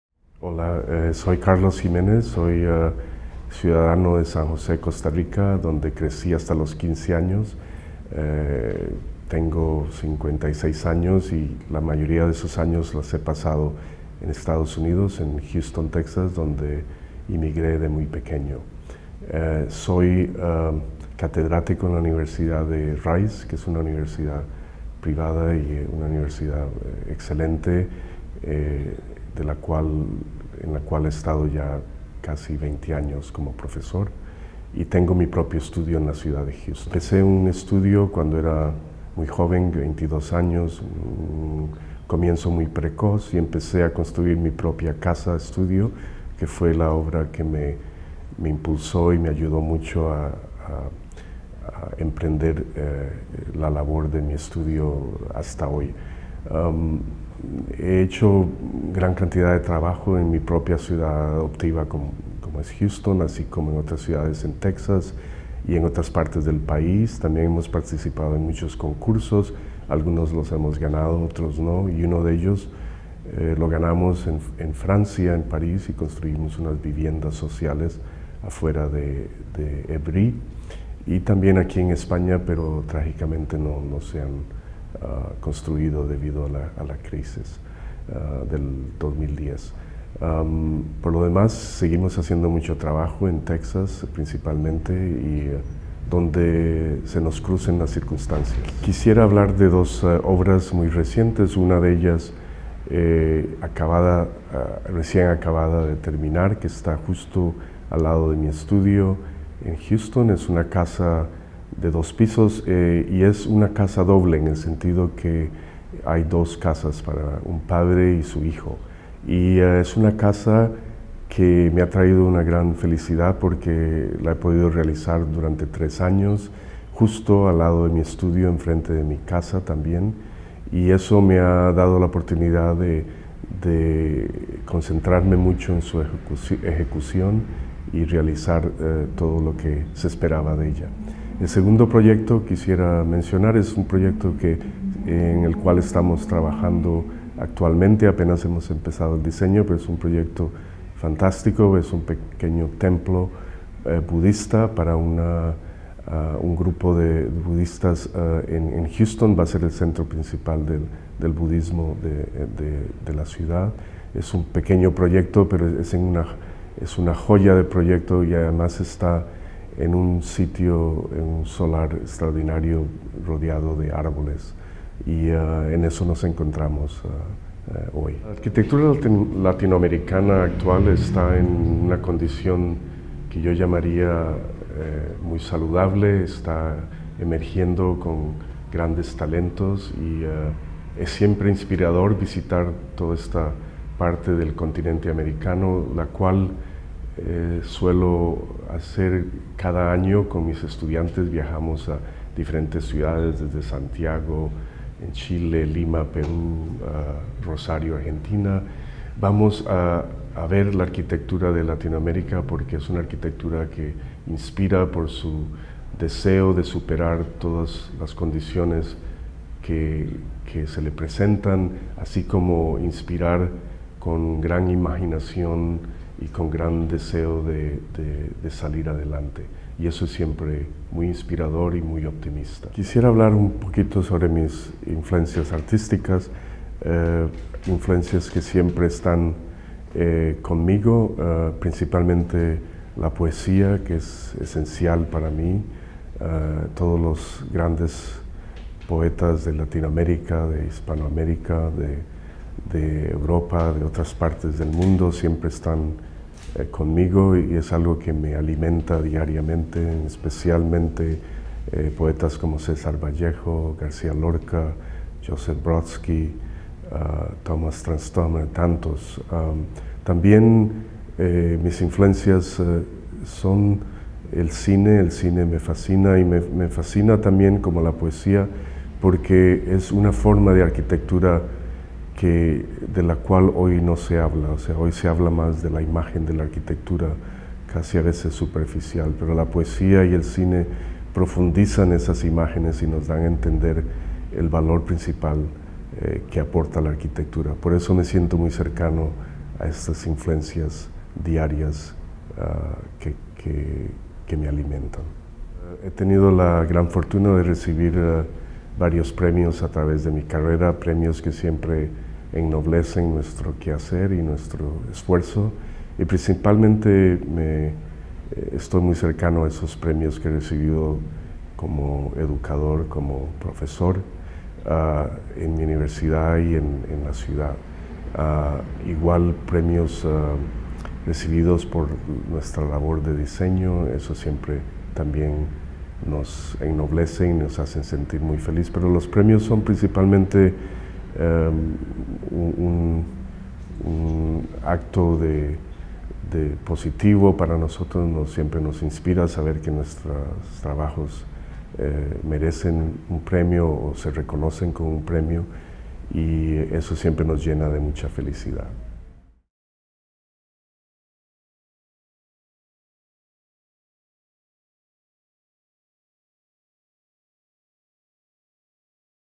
Carlos Jiménez - Entrevista
26/5/15. Entrevista al arquitecto costarricense.